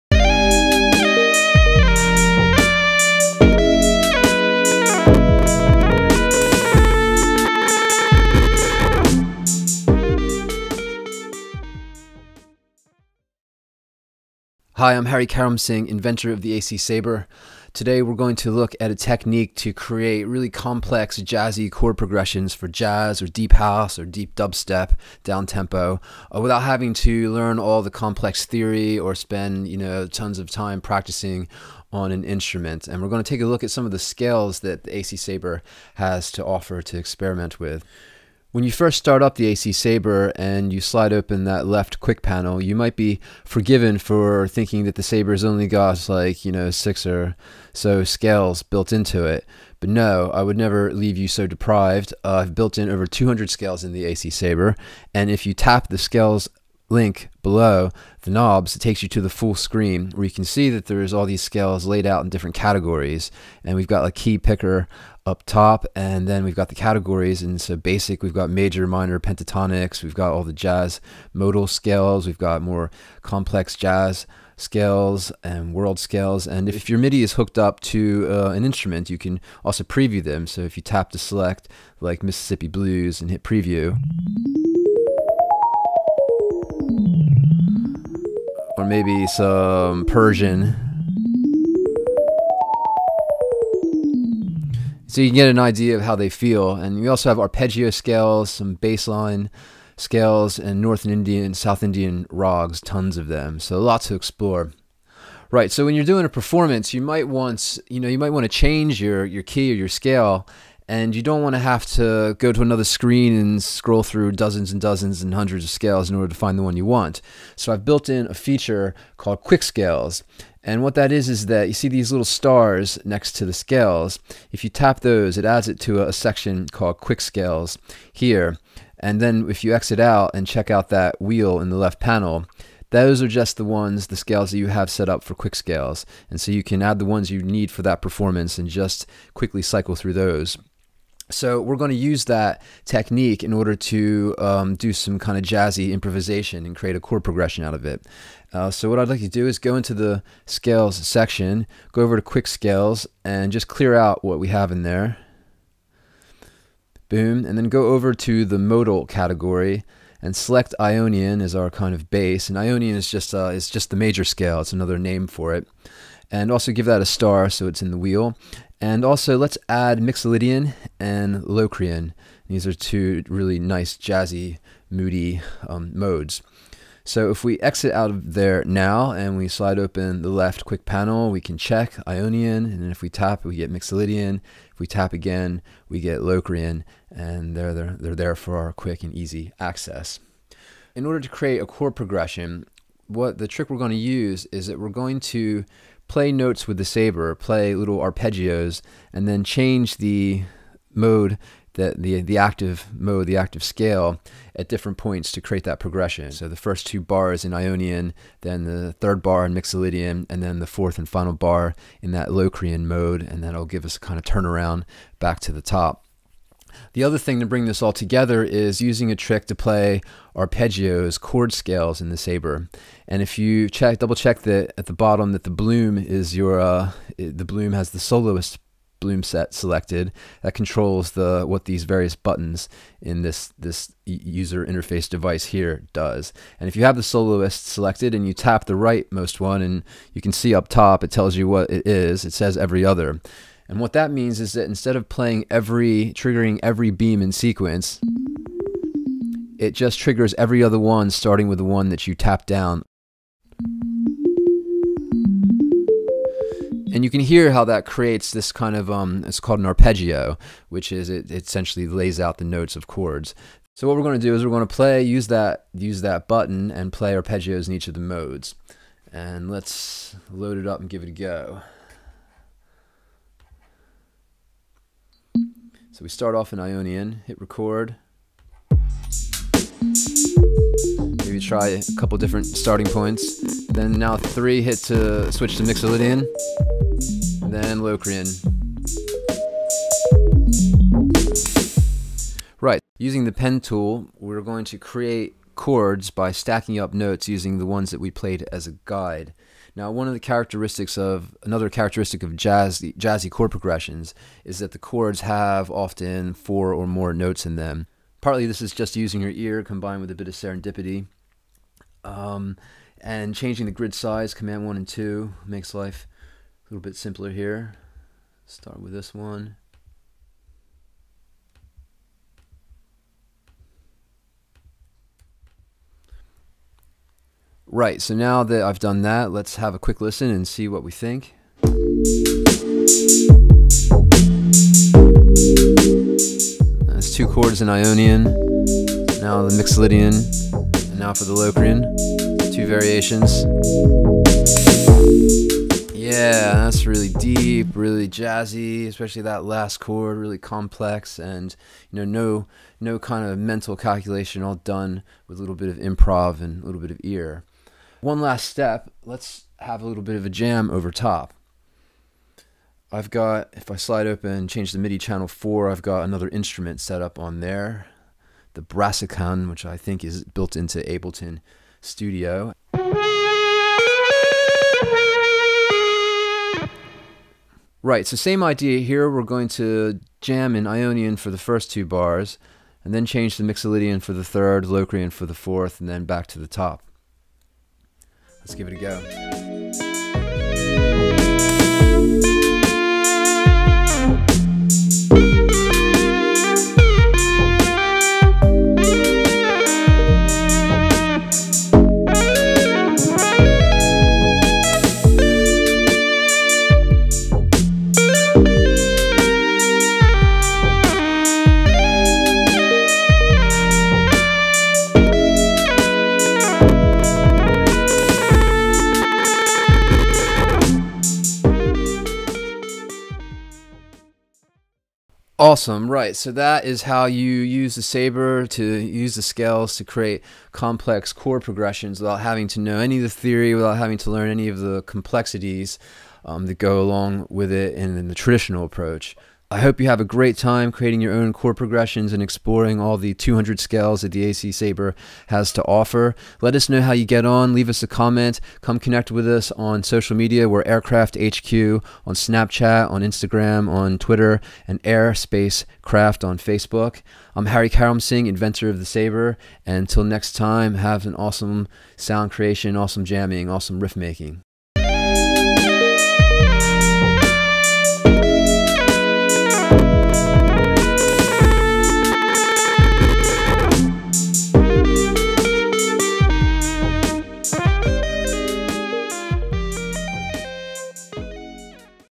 Ableton + AC Sabre 3: Deep Jazzy Chord Progressions, No Theory Required
— AC Sabre is a wireless, motion-based MIDI instrument that runs on your iPhone/iPod/iPad and is pretty handy for coming up with slick riffs and hooks for your productions and for live jamming on your synths like you're Hendrix. In the last two articles we looked at using the AC Sabre to do some [live wobble bass] and for some [energy pumping ARP-synth improvisation]  In this third and final part we're gonna deepen the vibe and use the Sabre as a composition tool to create jazzy chord progressions for deep house, jazzy dubstep, balearic, downtempo, etc. (Full article